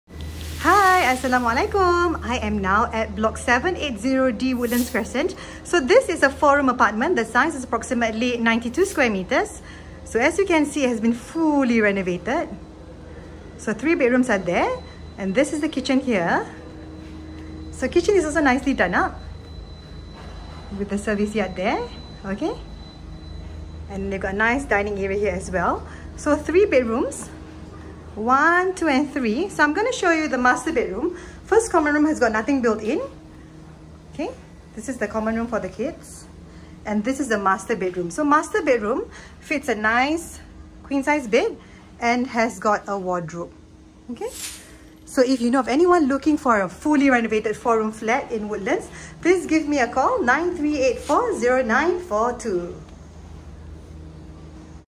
nonaejenrumah HDB For SALE! @ 780D Woodlands Crescent
Lease started 1 July 2015. Low floor corner unit. 4A Model. Approximately 92sqm. Fully renovated. Move in condition.